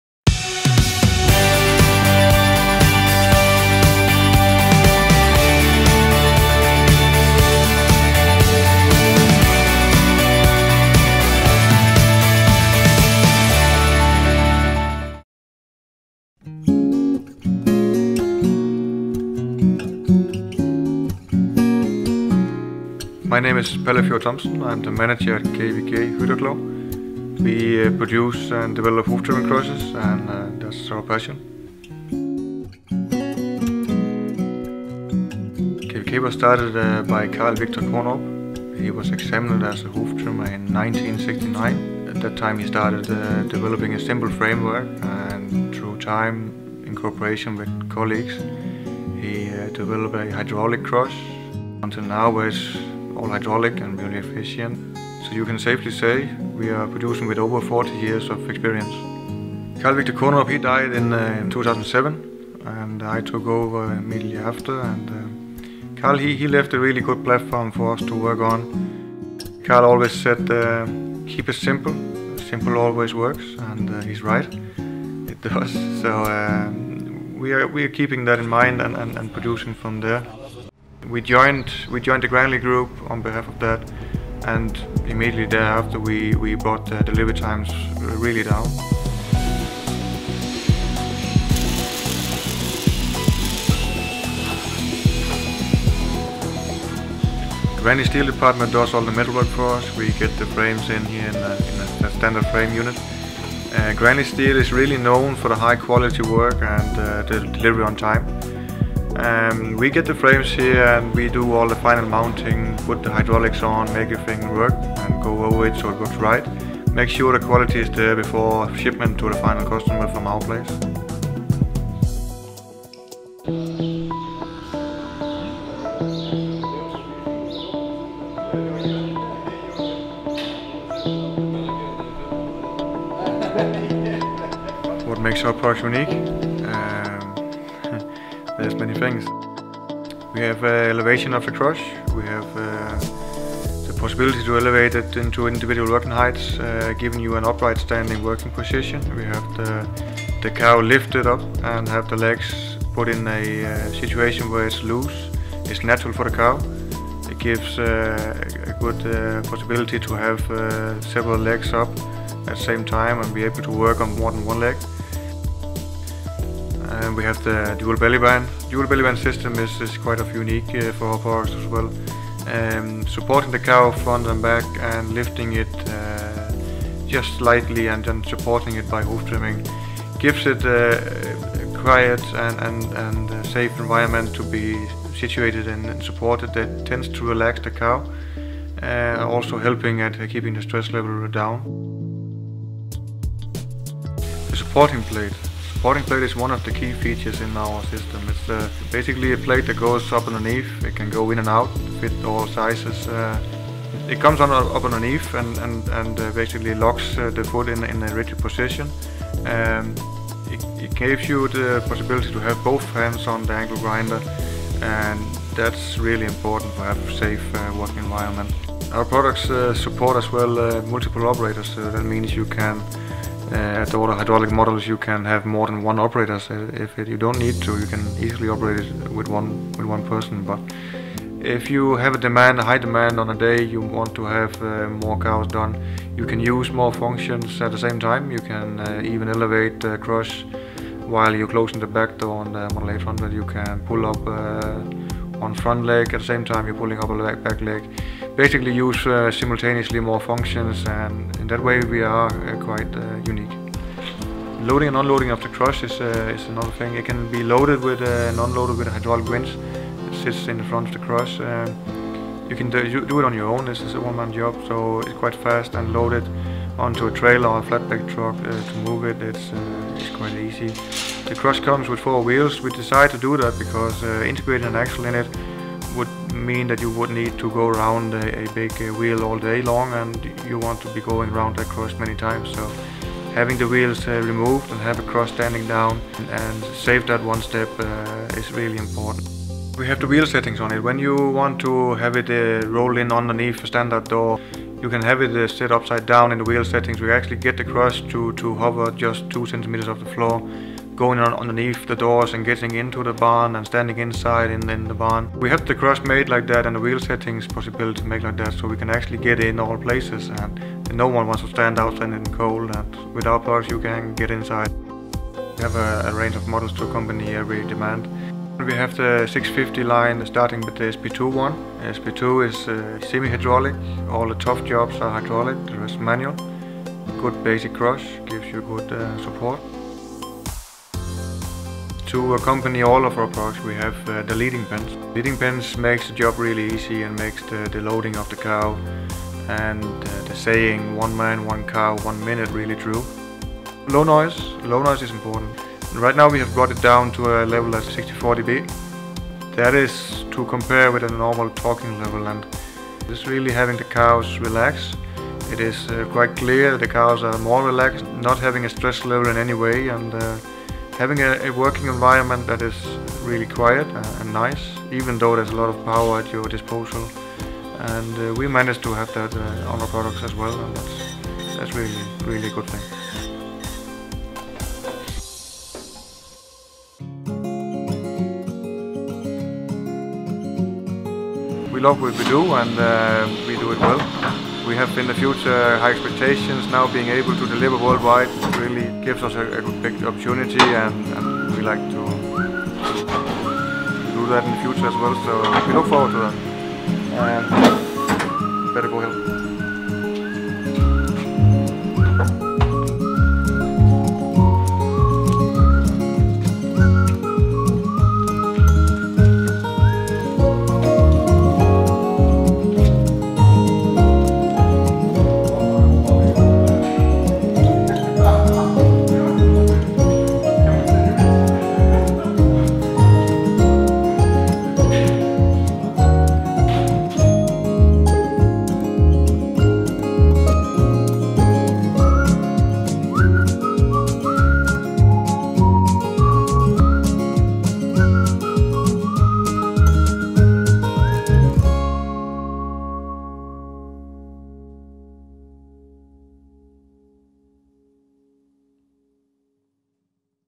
KVK Hydra Klov - Hoof Trimming Chutes
00:17 - 02:03 Background and production
02:03 - 06:17 Different models and technologies
06:17 - 07:30 Outtro

KVK Hydra Klov is a modern company who develop and produce hoof trimming chutes and accessories for professional hoof trimmers and farmers worldwide. This video provides insight in the production, the technology and the overall spirit of KVK Hydra Klov.